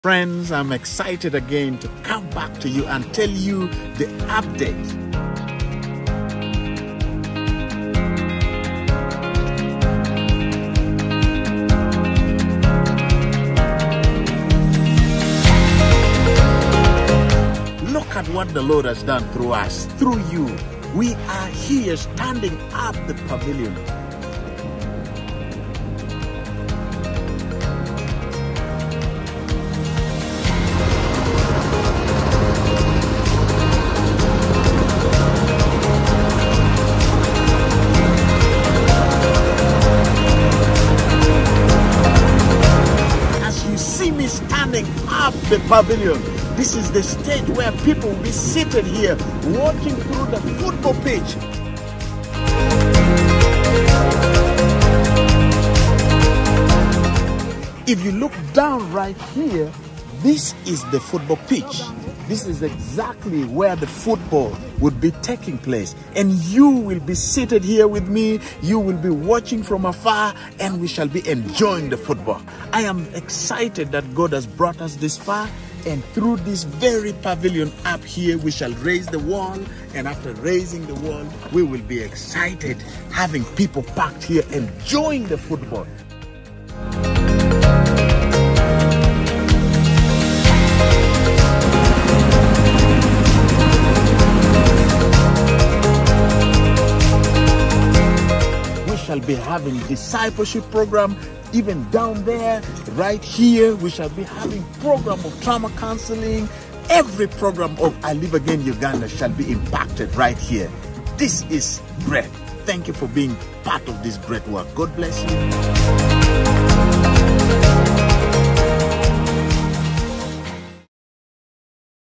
The Latest Update from I Live Again Uganda's - The Potter's House!
Check out this latest update from Uganda!
Buildings are coming up and the football pitch is ready for some grass!